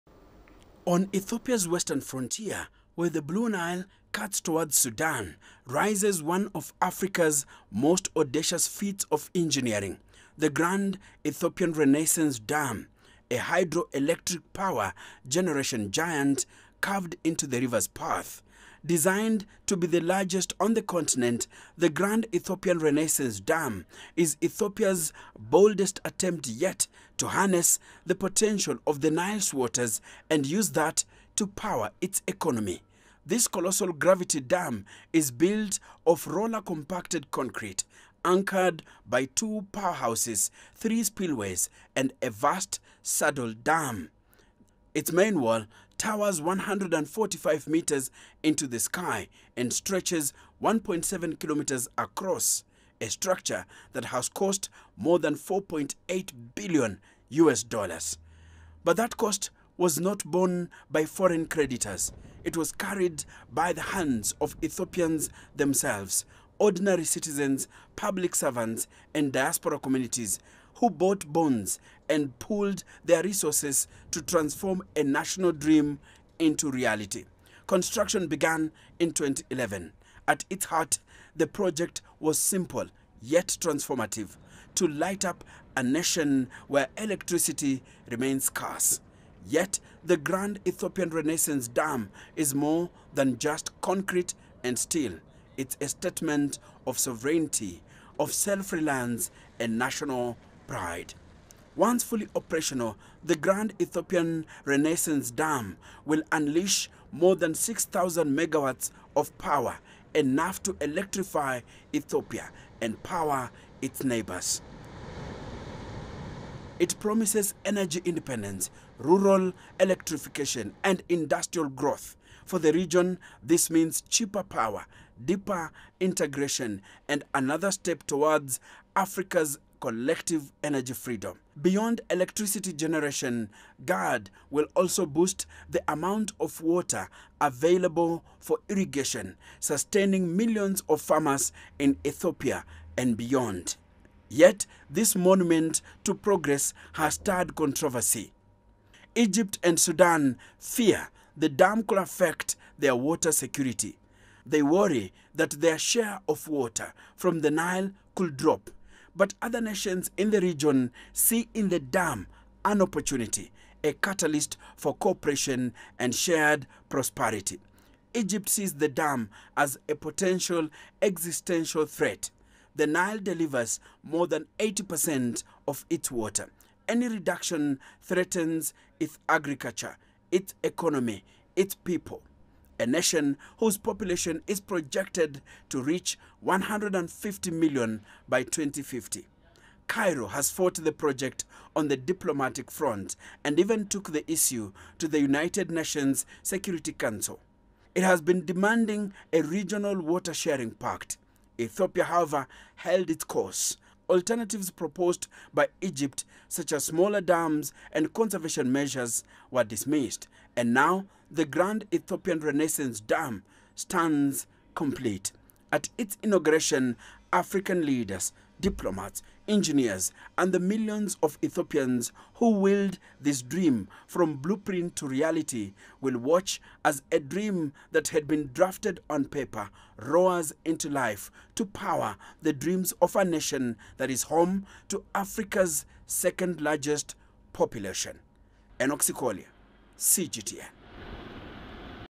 Ethiopia to inaugurate Africa’s biggest hydropower dam on the Nile
Towering over the Blue Nile, the Grand Ethiopian Renaissance Dam is Africa’s largest hydroelectric project. Built with homegrown funding, this $4.8 billion megastructure will generate 6,000 MW of power. As Ethiopia prepares for its official inauguration, the dam stands as a symbol of national pride, resilience, and regional ambition.